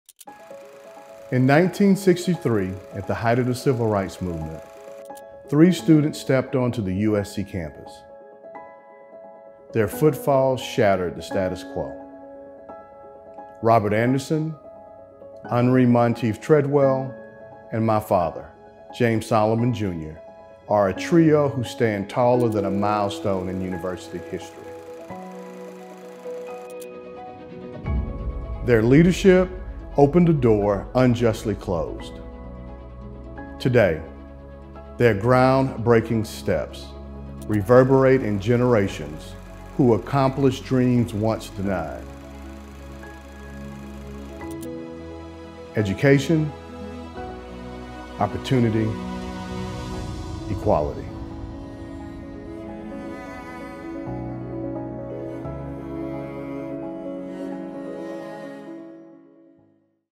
USC unveils monument honoring desegregation trailblazers
Today, the University of South Carolina unveiled a monument honoring the first Black students admitted since Reconstruction — Robert Anderson, Henrie Monteith Treadwell and James Solomon Jr. — whose enrollment six decades ago changed the course of university history.